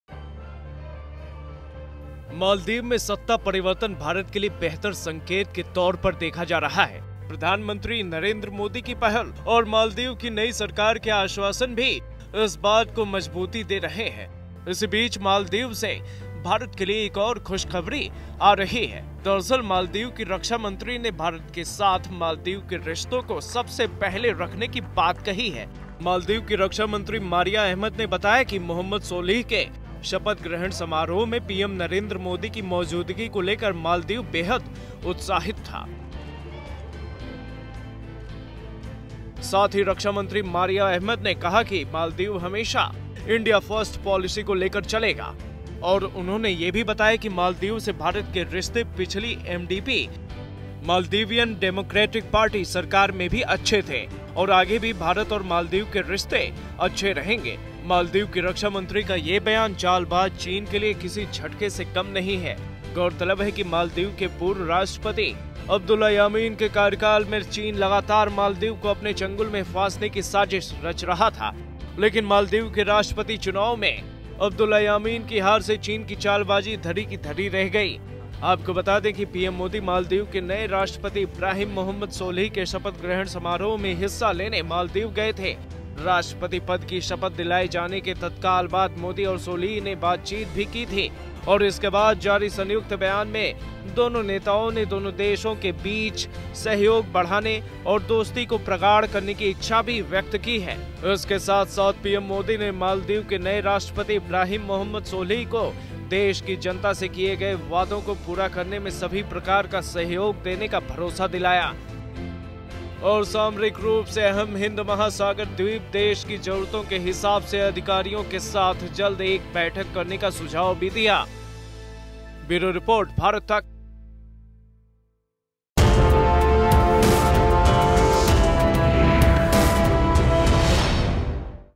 जिनपिंग को मालदीव ने दिया एक और झटका | Bharat Tak
मालदीव में सत्ता परिवर्तन भारत के लिए बेहतर संकेत के तौर पर देखा जा रहा है.प्रधानमंत्री  नरेन्द्र मोदी की पहल और मालदीव की नई सरकार के आश्वासन भी इस बात को मजबूती दे रहे हैं ।